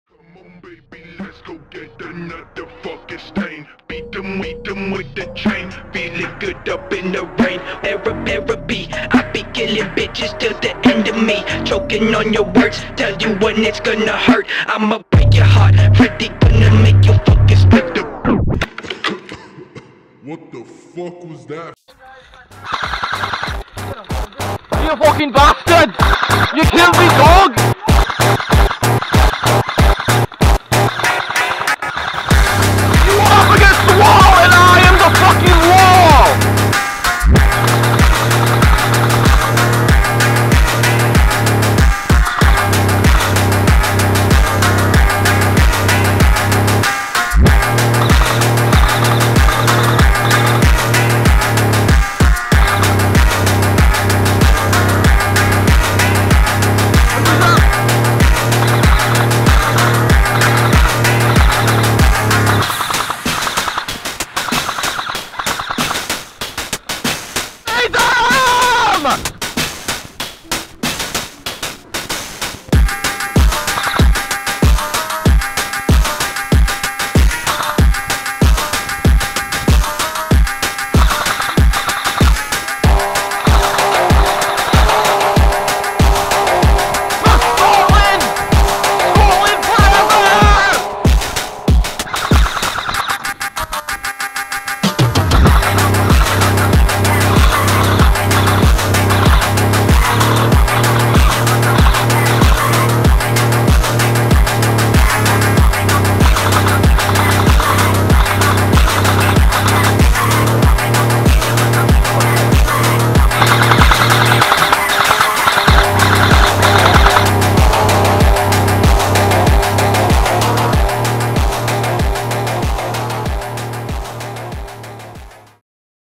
Airsoft skirmish funk music montage [Code Red] Team Shogun
I don't know what this is but I tryed doing a military edit video with my footage